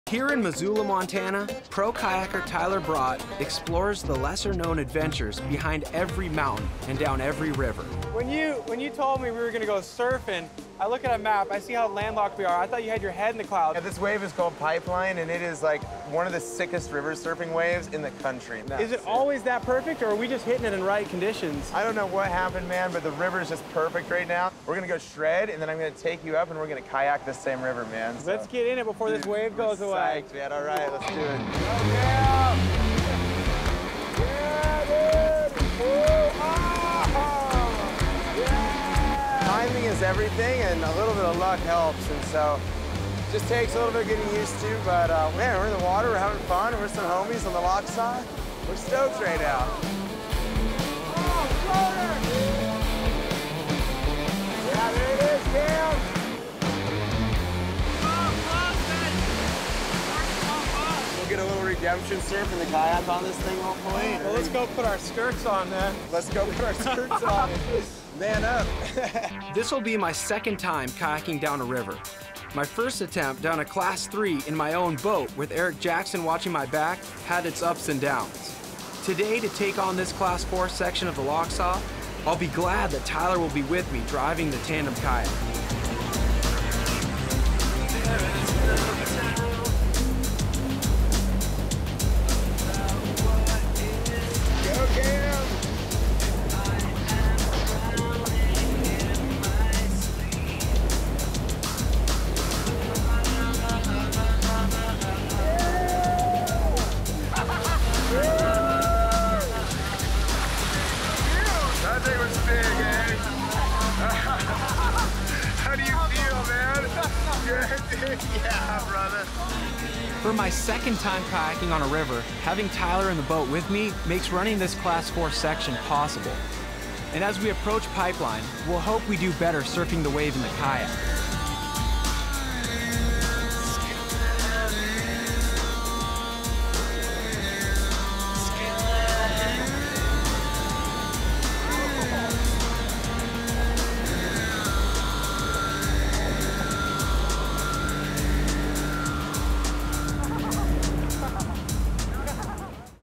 Tyler Bradt takes Cam McCaul Kayaking | Locals
Pro kayaker Tyler Bradt, welcomes Cam McCaul to Montana with some river surfing and standing wave kayak surfing.